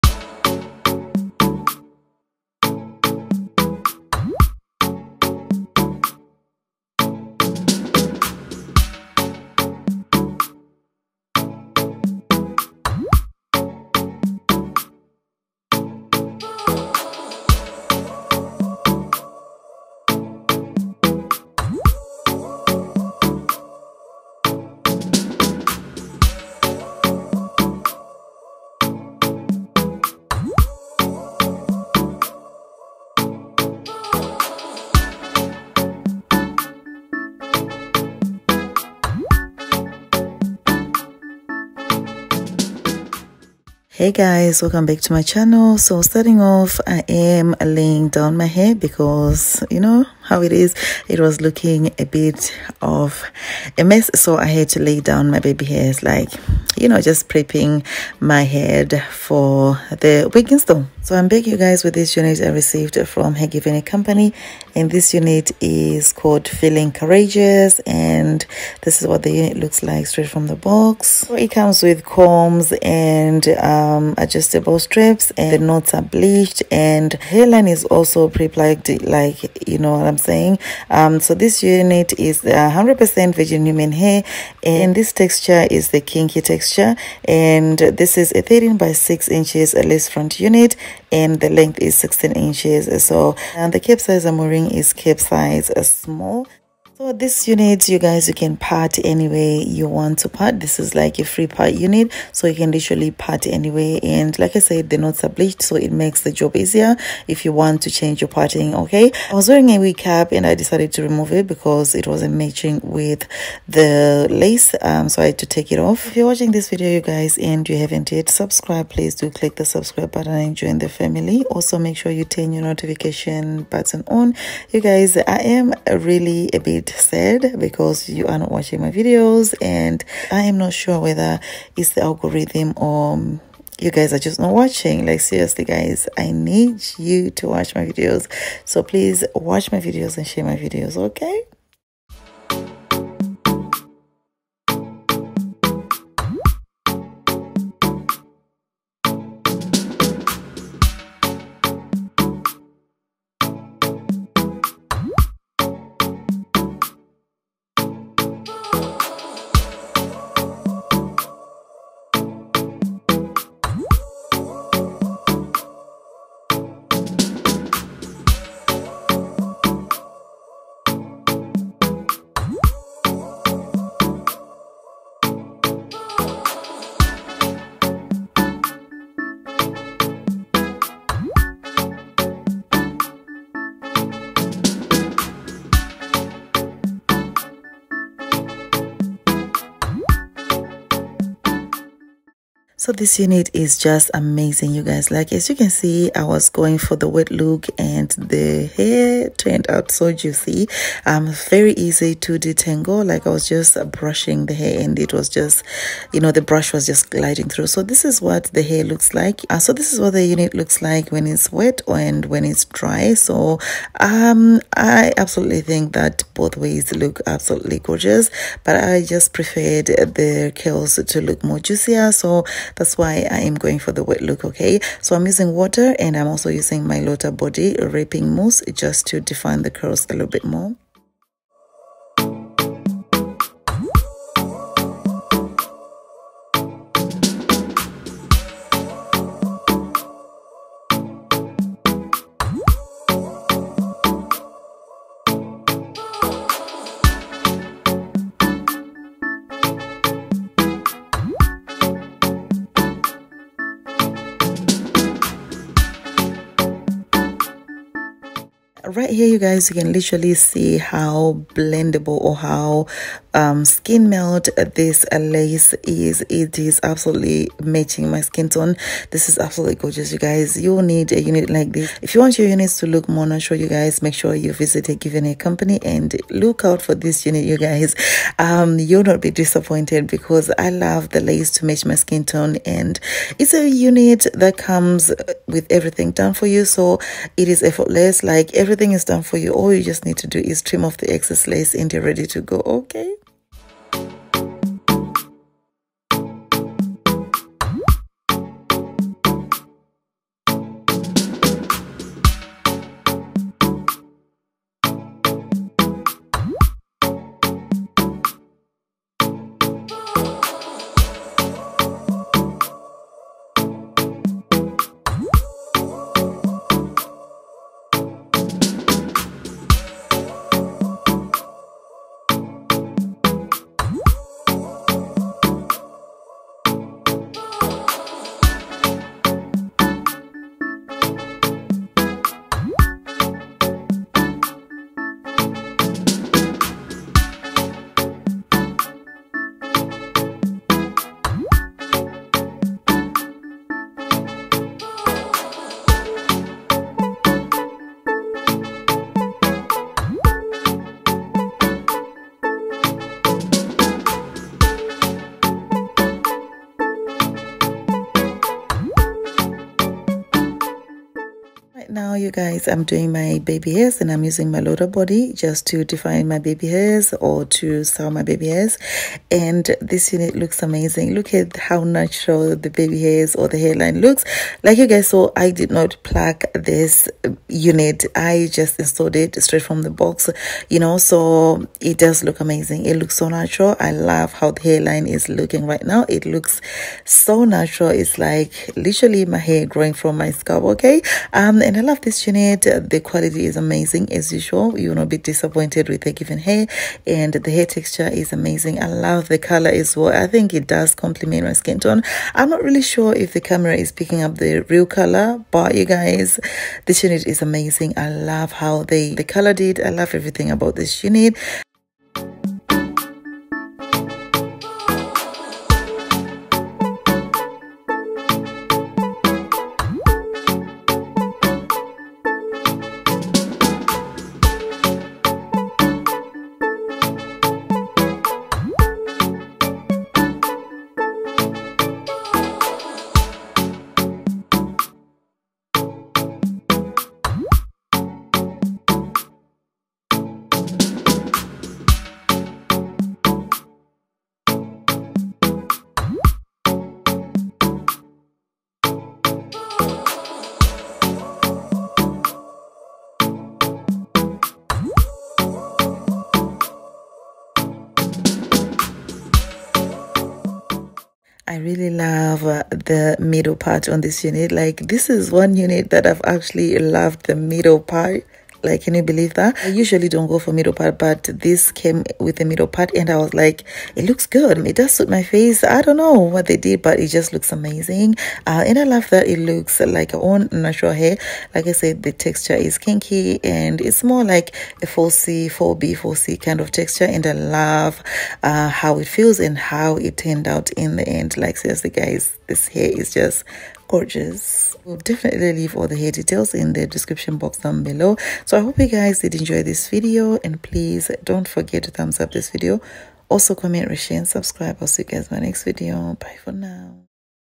OMG 🔥 GIRL IS THAT YOUR REAL NATURAL HAIR? |SKIN MELT NATURALHAIRWIGS | SCALP ✅ Hergivenhair
Hi my loves am back with this OMG 🔥 GIRL IS THAT YOUR REAL NATURAL HAIR? |SKIN MELT NATURALHAIRWIGS | SCALP ✅ Hergivenhair This naturalhairwigs is so perfect for a protectivestyle it blends beautifully and looks so much like
HERGIVENHAIR

Wig Name: Feelin Courageous-TLW97S
Hair Texture: Kinky
Wig Type: Lace Front Wig (13"*6")
Hair Length: 16 inches
Capsize: Small

FAQ:
Pre-plucked Hairline - Yes
Deeply-bleached Knots - Yes
Swiss Transparent Lace - Yes
100% Virgin Human Hair - Yes
Adjustable Elastic Band - Yes

I TRIED RUBBER BAND BOX BRAIDS HOW TO GRIP ROOTS BOX BRAIDS